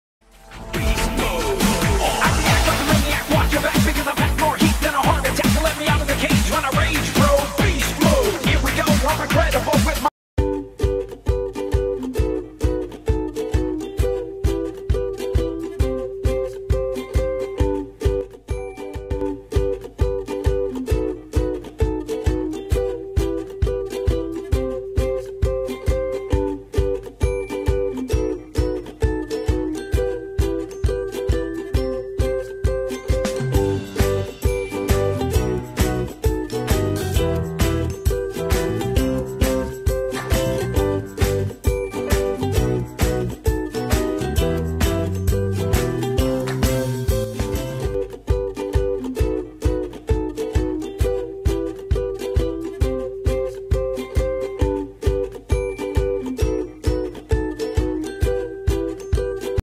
Delivering pizza for my guinea pigs!!
They really loved this pizza!